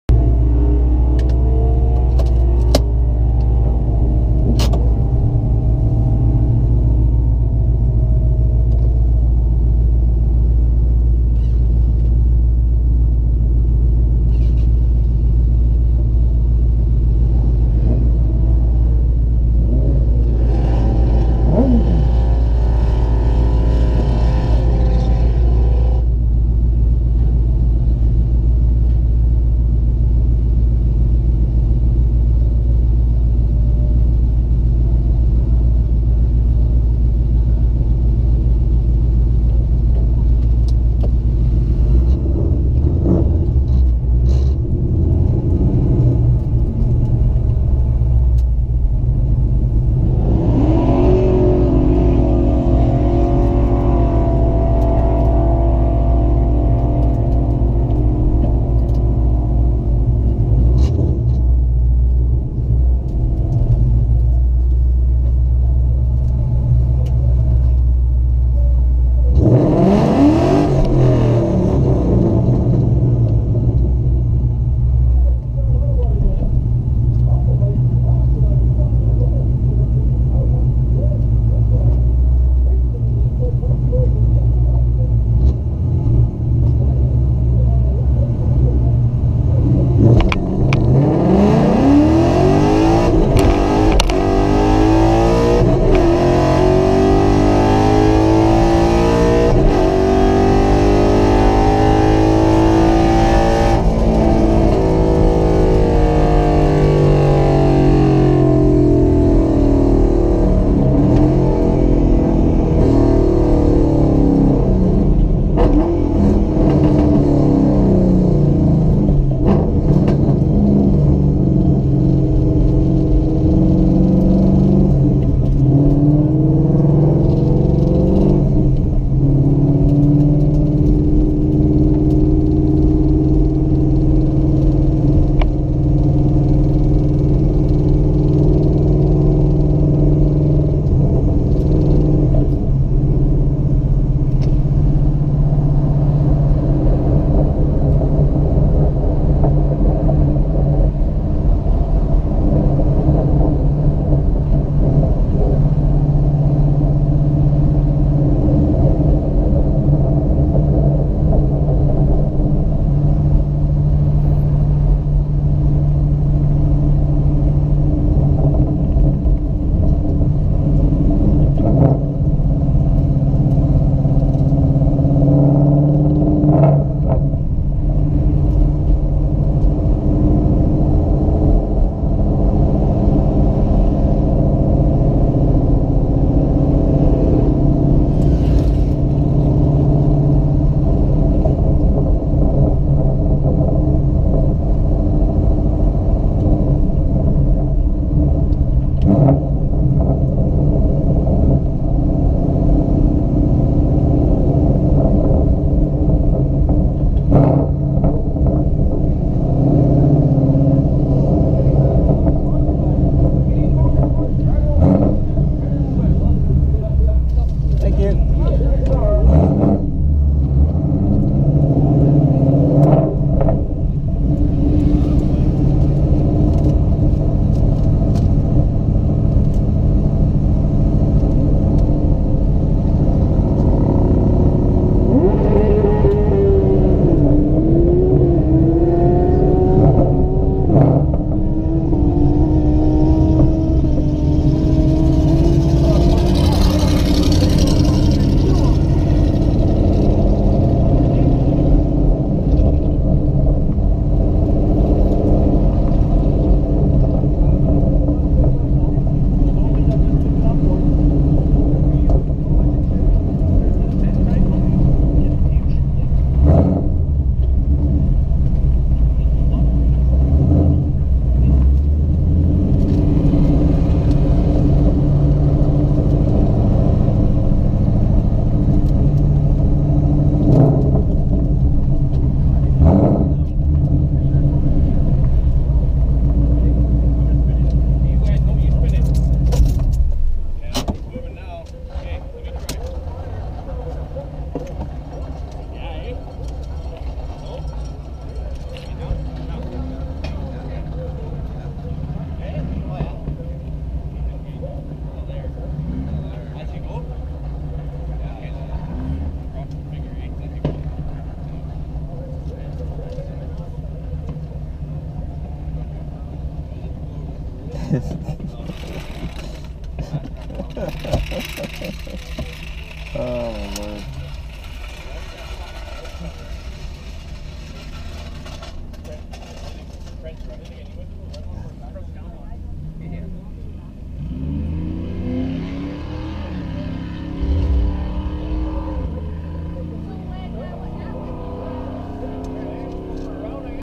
Test and Tune August 3rd, 2019 @ Interlake Dragway near Gimli
First time out at Gimli and having a blast getting to learn the track and meeting new people.  Was a hot day, but lots of neat vehicles and types of all classes were out there having fun!  I pedalled 2nd gear since I felt it kick out and I'm not use to the track yet.  Other than that, had a pretty good run of 12.89 on stock tires which are really damn old that this point.